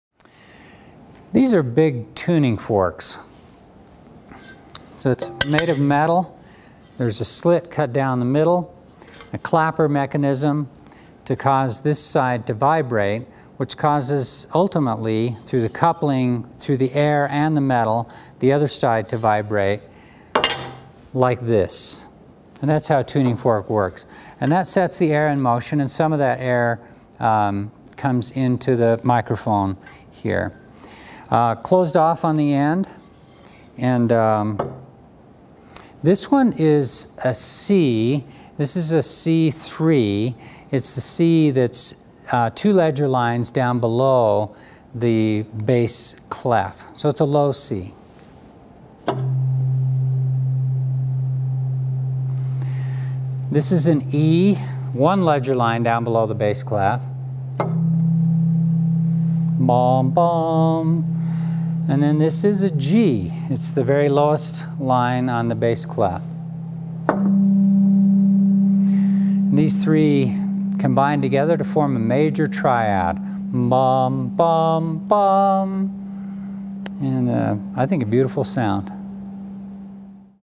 Major Triad Demo: Tuning Forks
This is a demonstration of a major triad produced by three tuning forks.